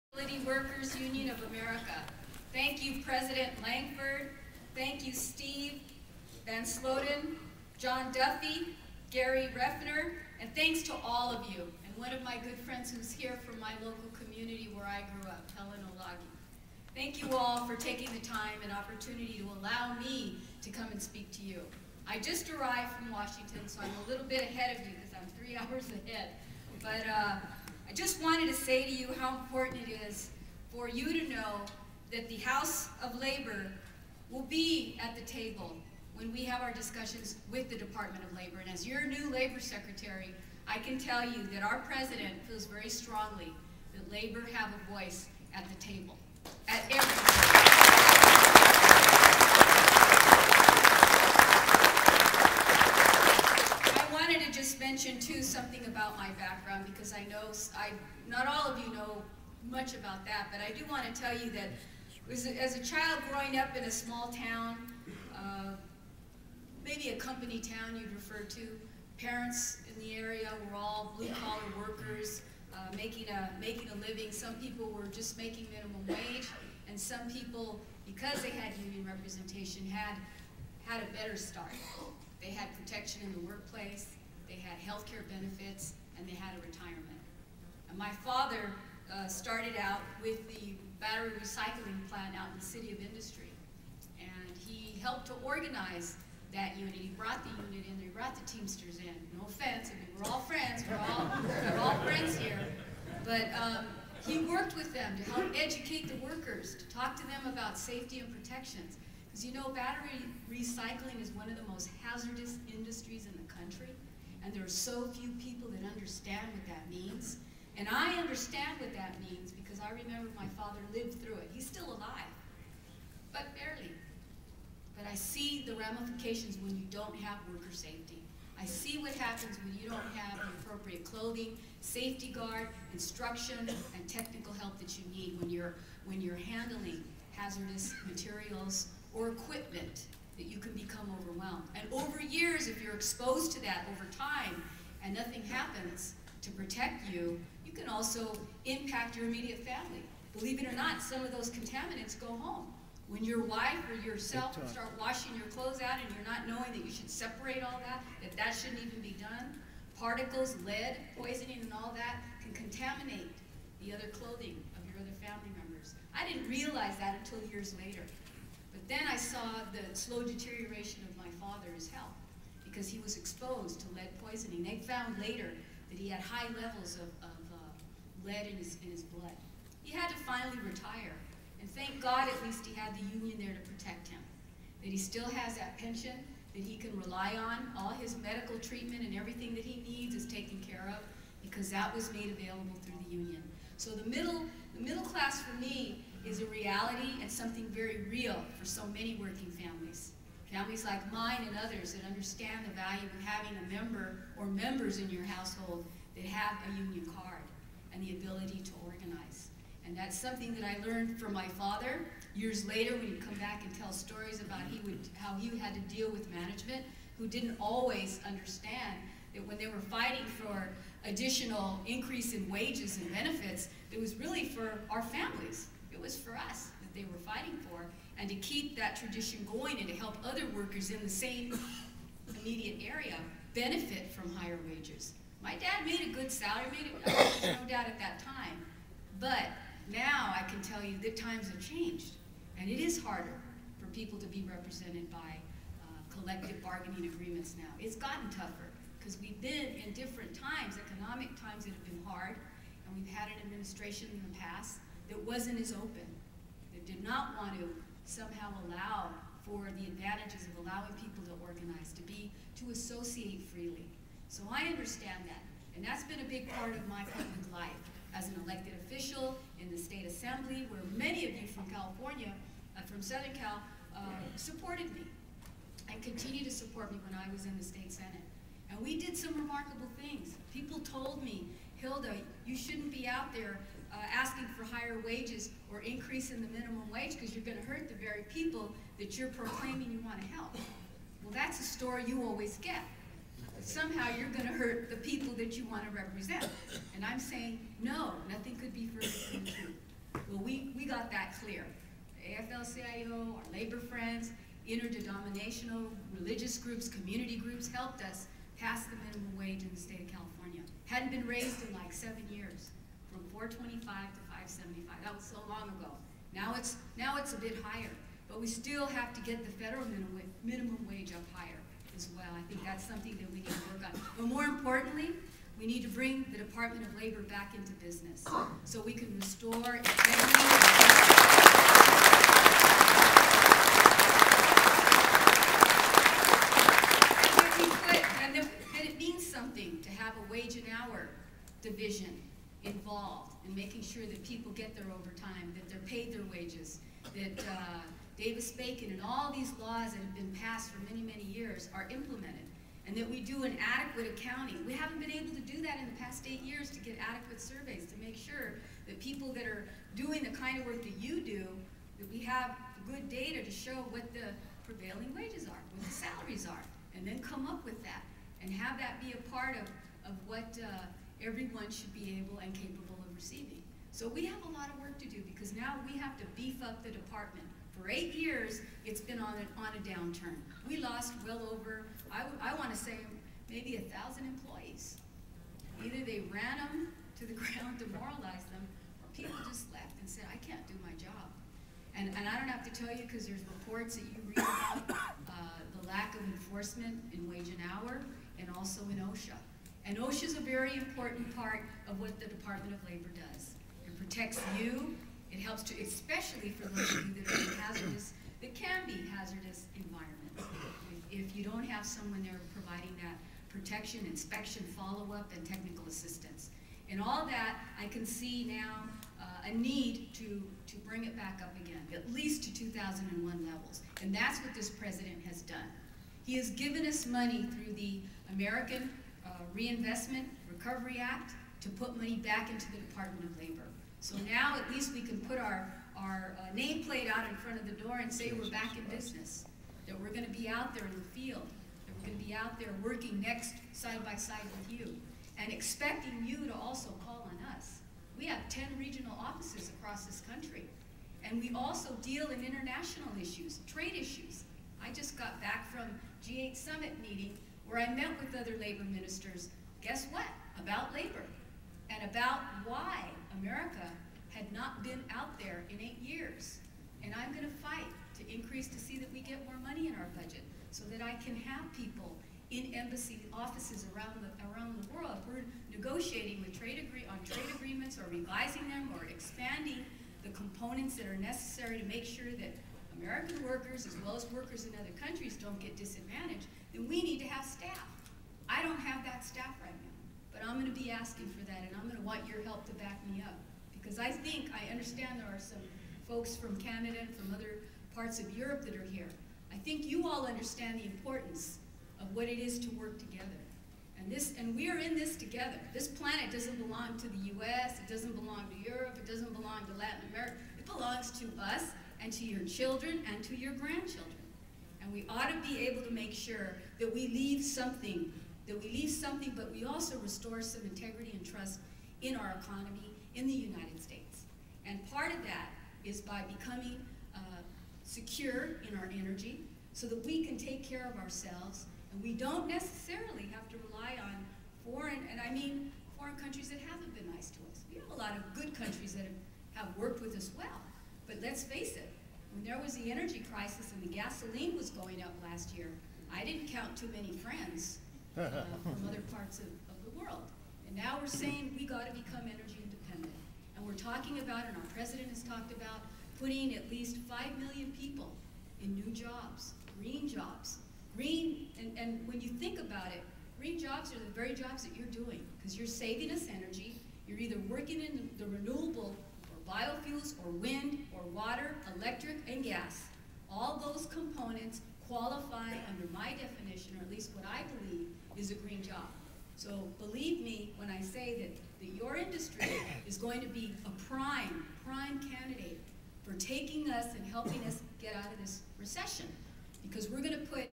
U.S. Labor Secretary addresses the U.W.U.A. part 1
U.S. Labor Secretary Hilda Solis addresses the members of the Utility Workers Union of America